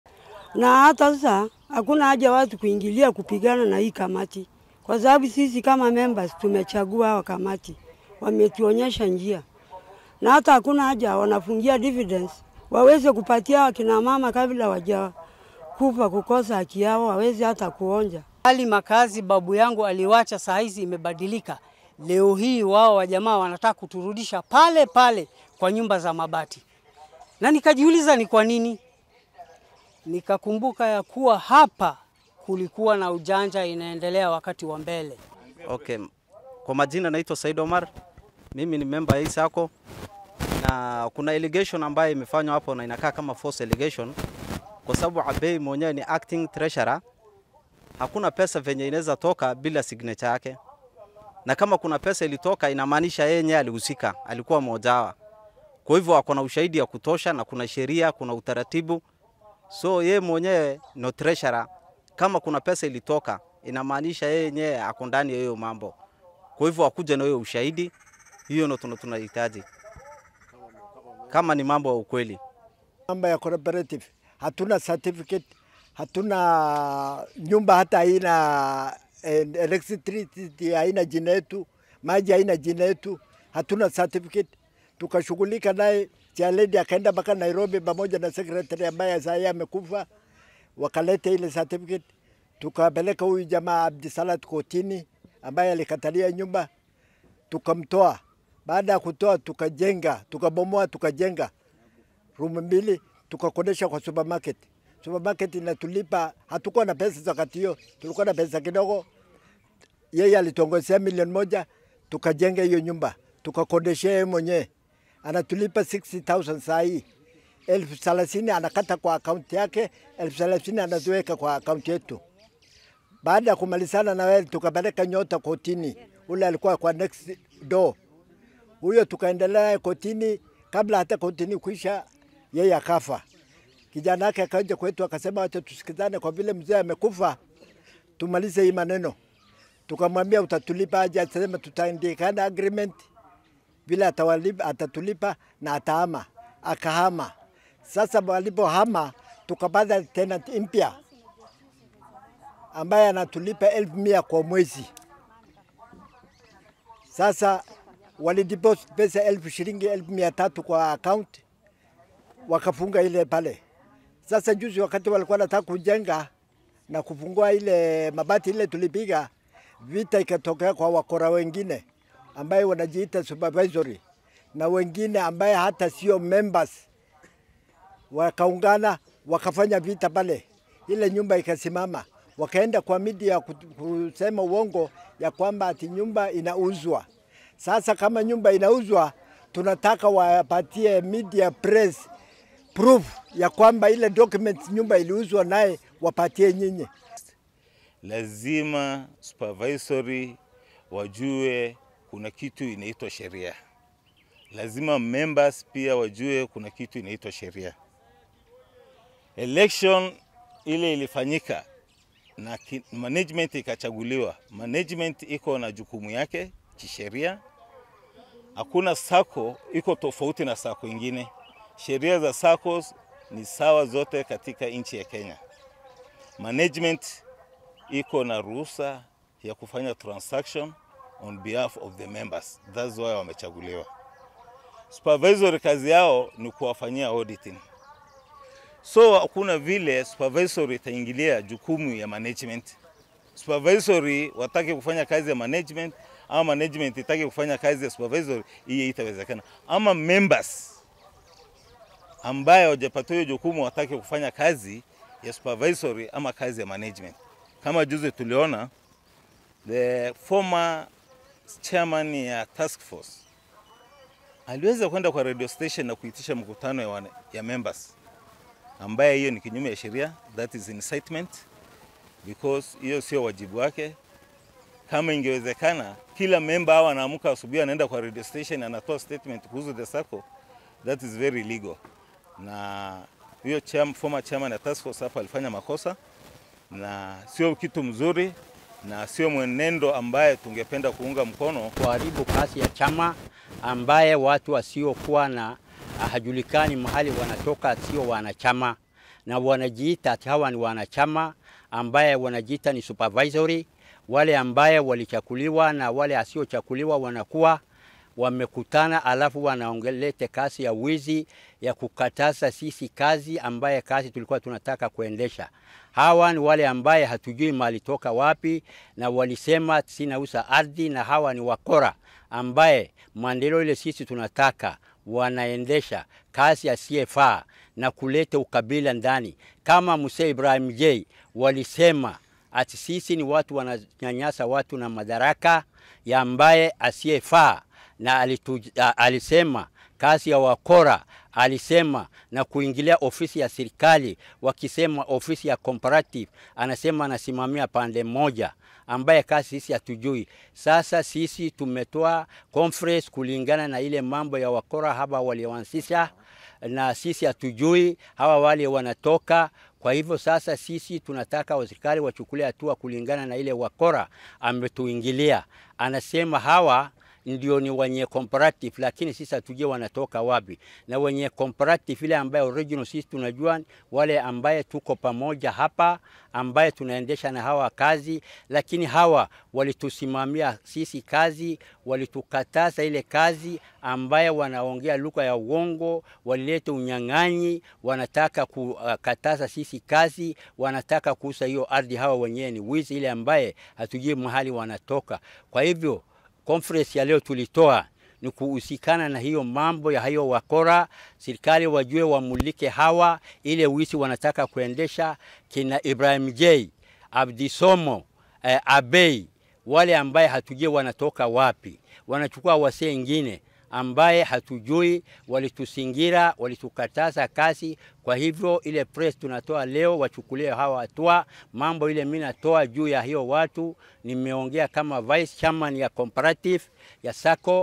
Isiolo prime plot that is creating division among Cooperative Members
There has been push and pull between two functions of the Isiolo farmers Cooperative